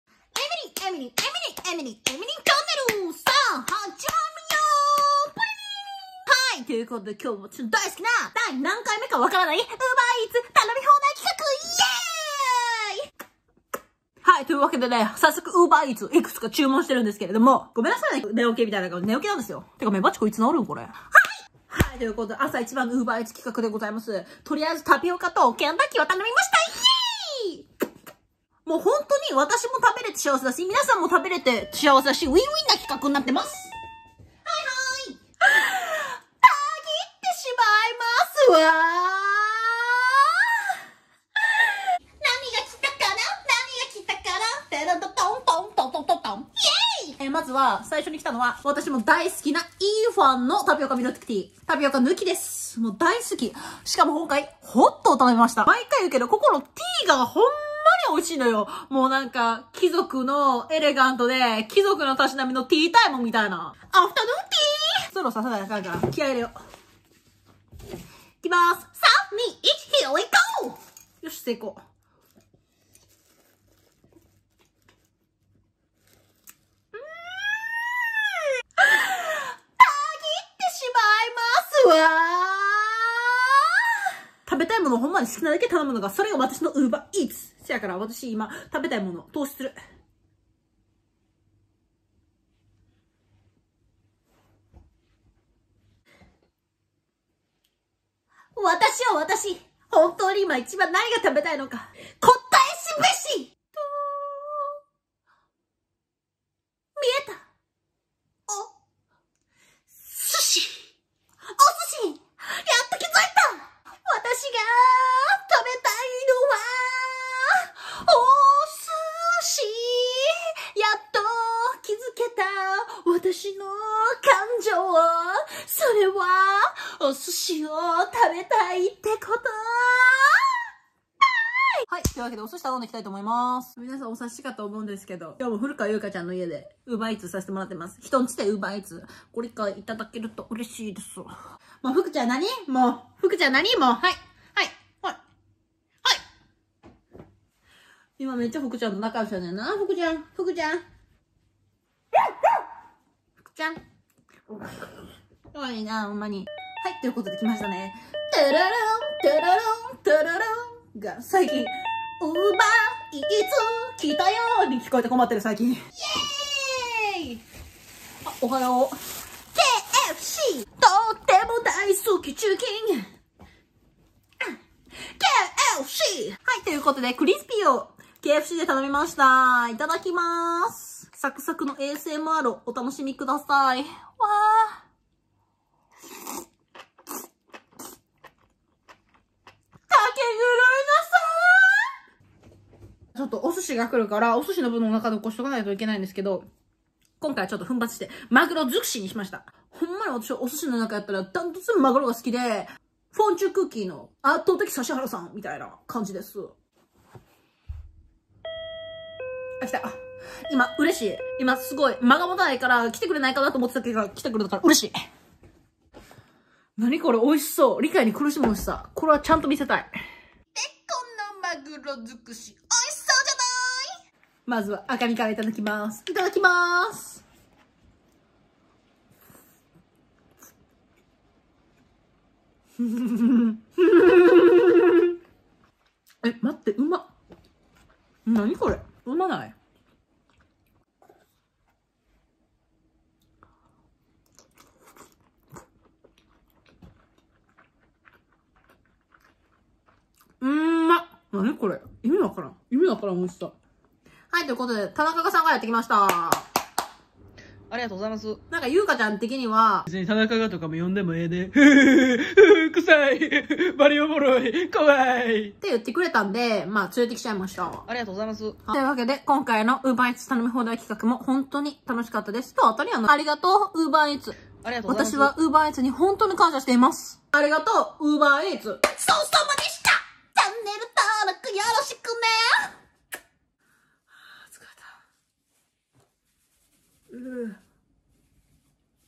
エミリンのものまねしたら見た目がペ・ヨンジュンになった【エミリンウーバーイーツ食べ放題パロディ】
えみりんのものまねさせてくれてありがとう🥺🙇🏻

丸山礼